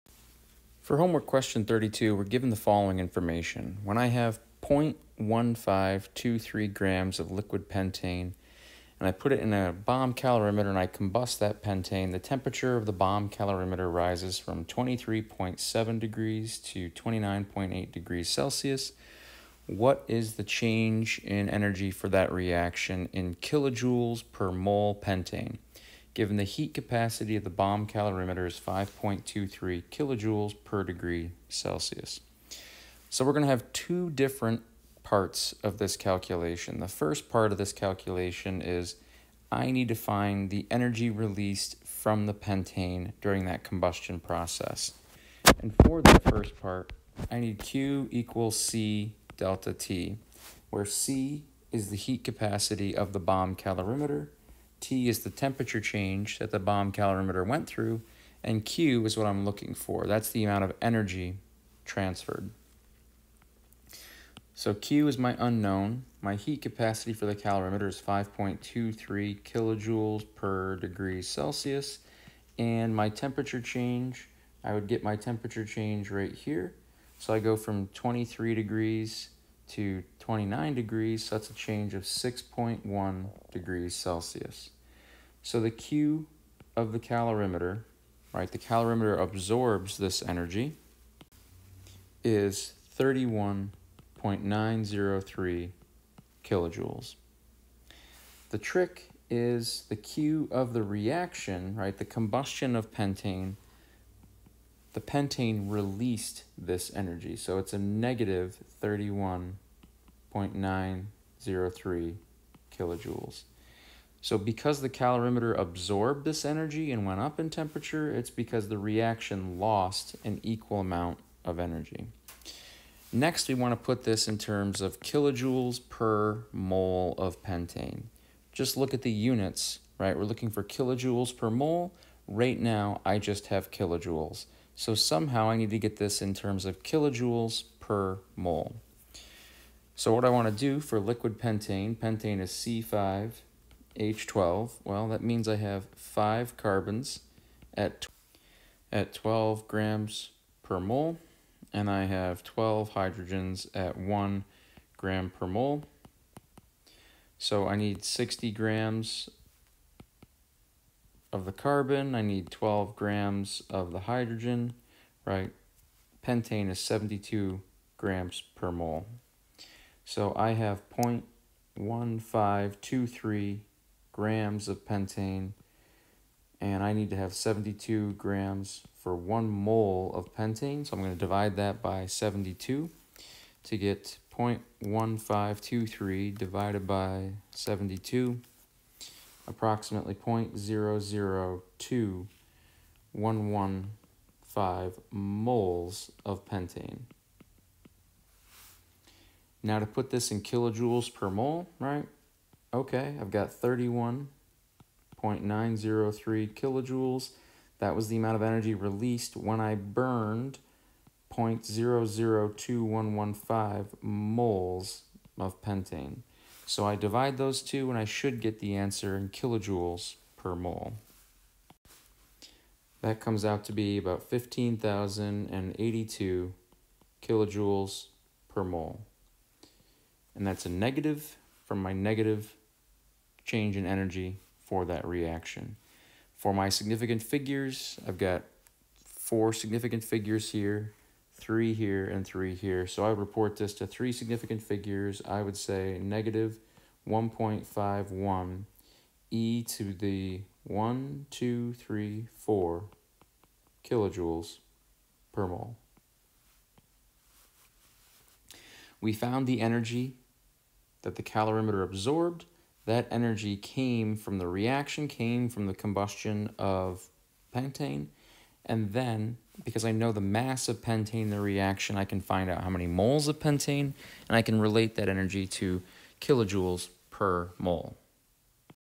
CHE-106-HQ5.32
Determining the change in energy of a reaction using bomb calorimetry